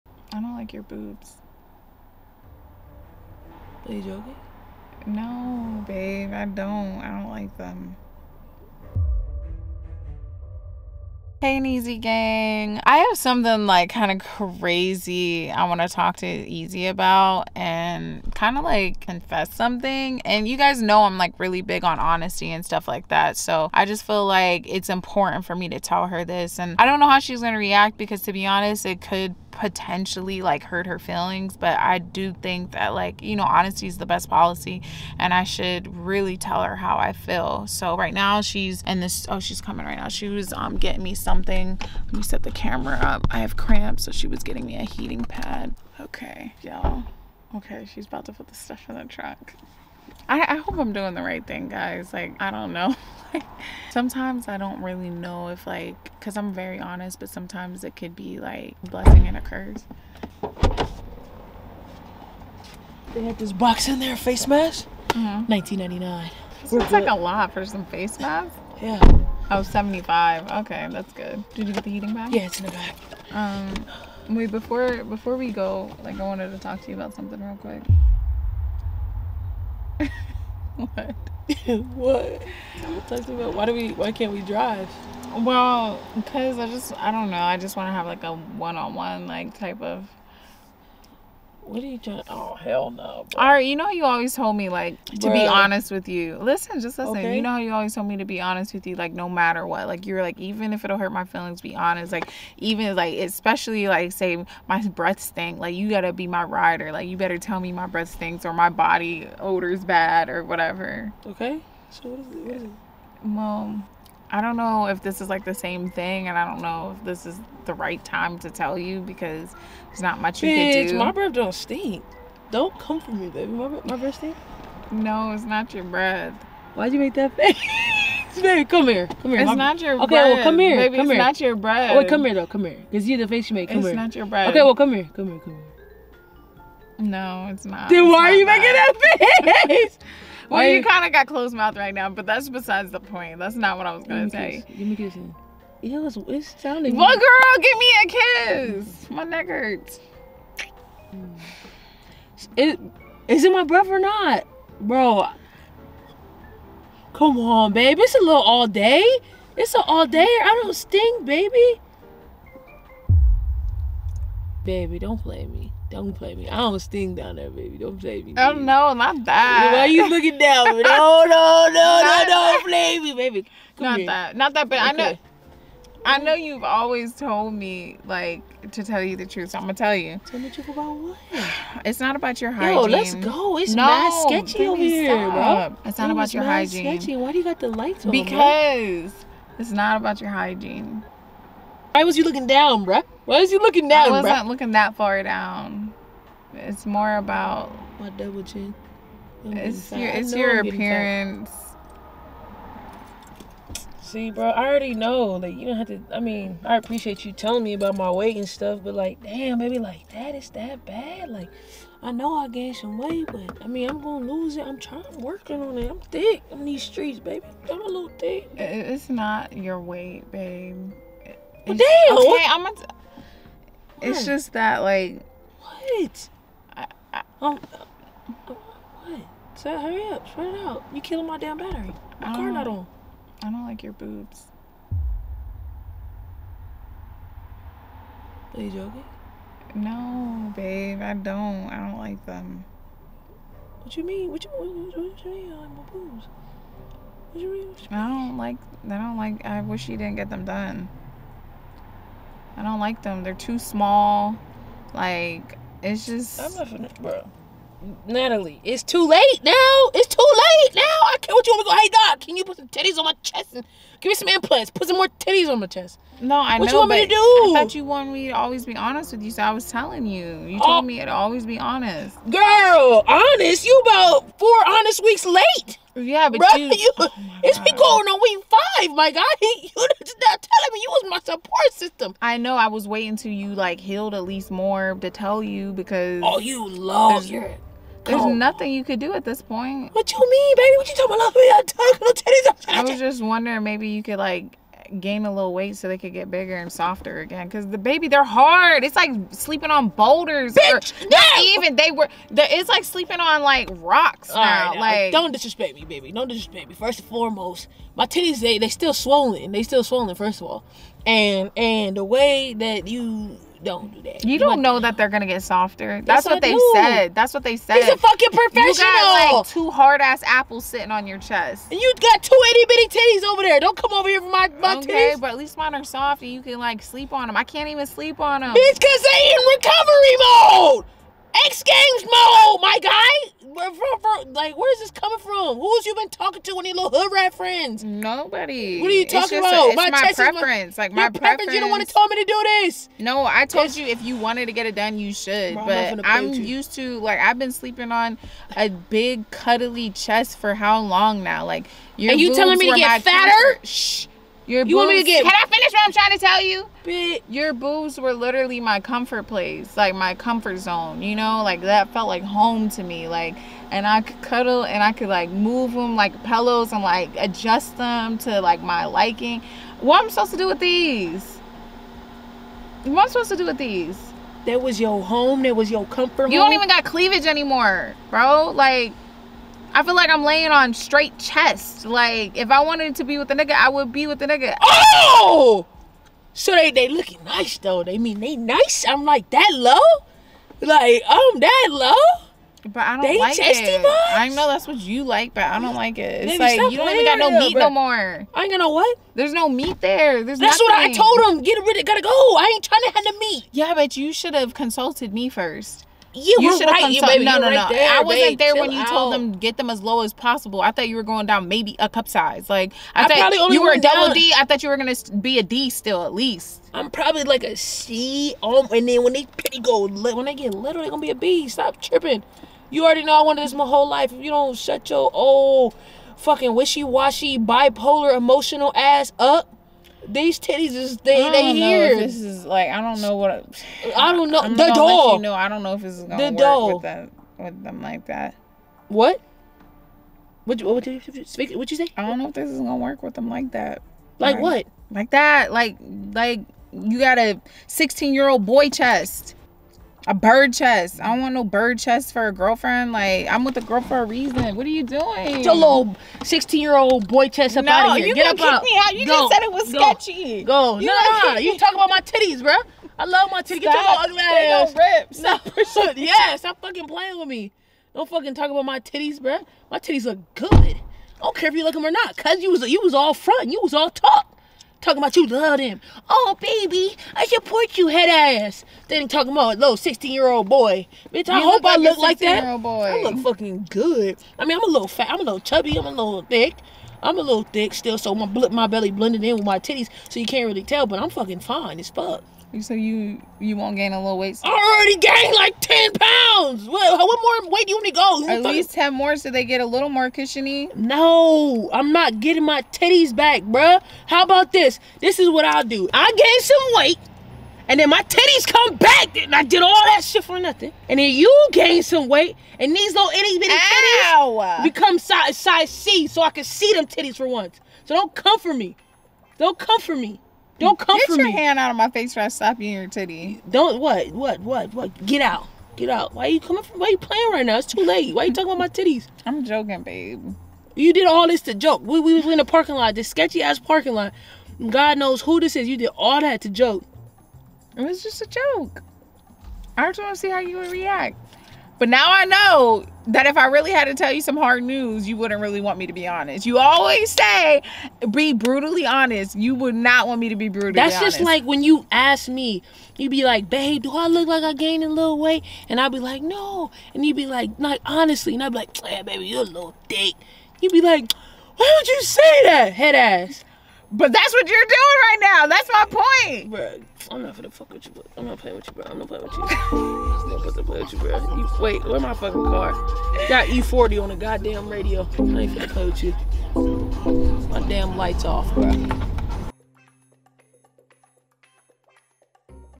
I DONT LIKE MY GIRLS BREAST SURGERY RESULTS! (THE TRUTH)
What do you all think?